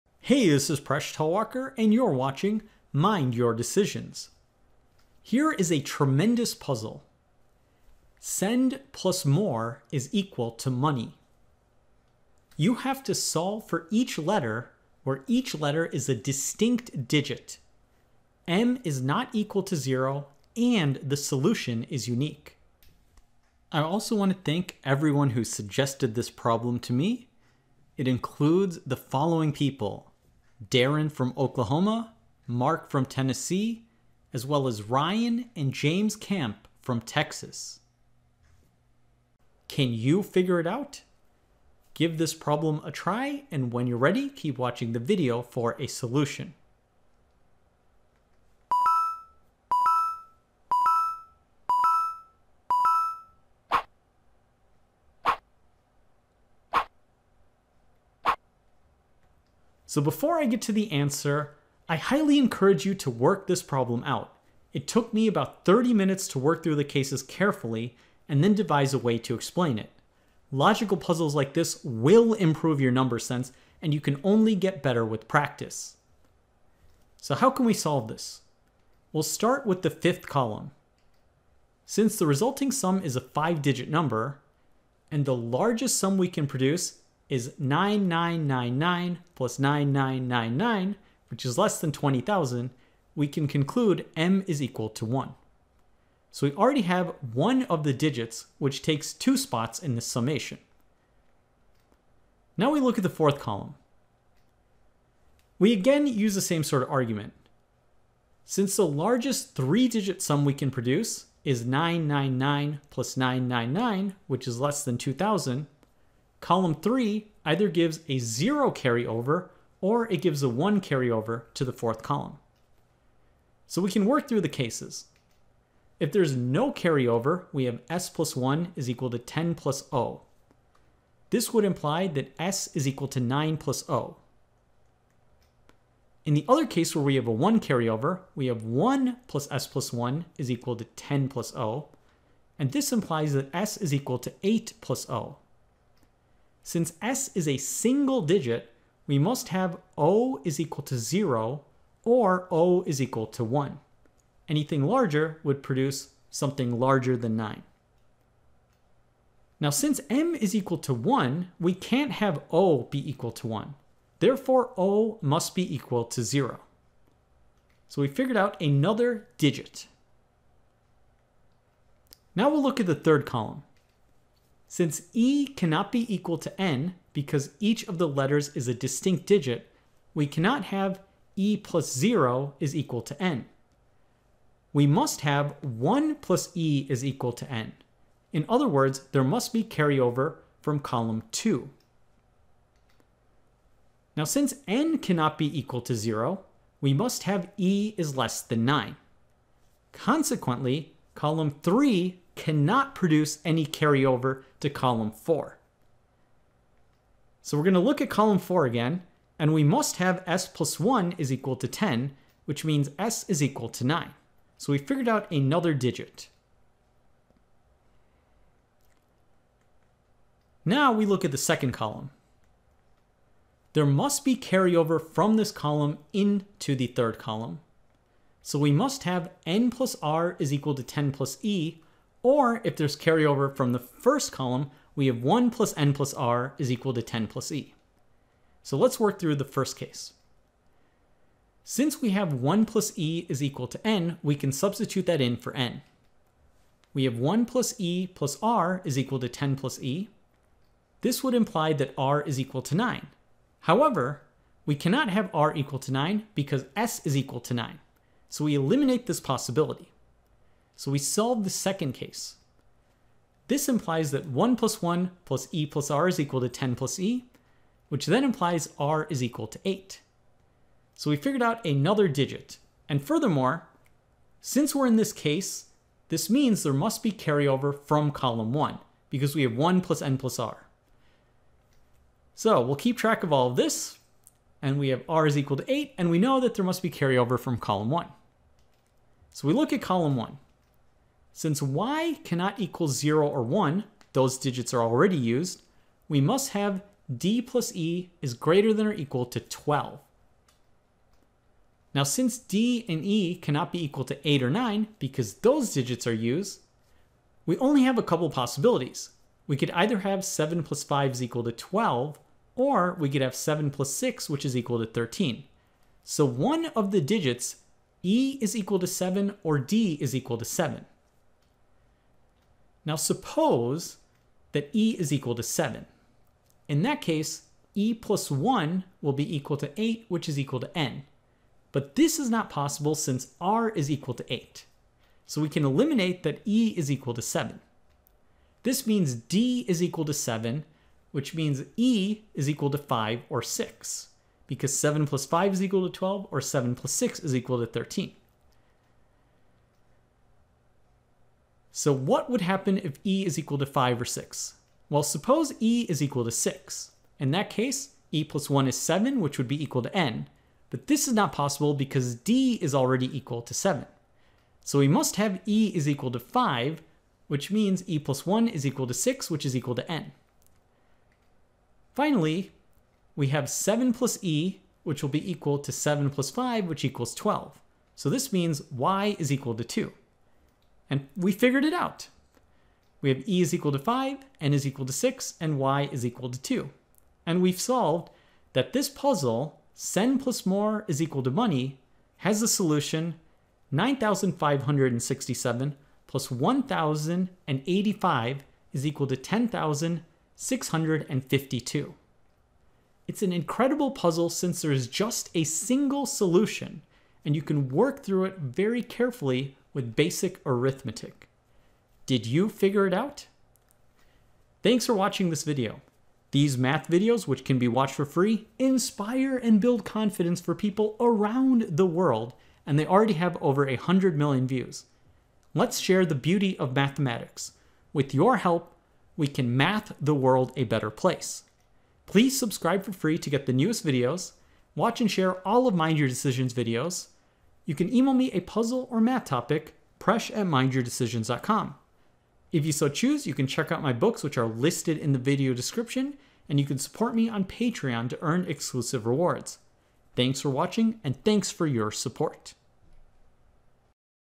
Only A "Genius" Can Solve - Each Letter Is A Number
What value is each letter? This is an incredible math problem with just a single solution! Can you figure it out?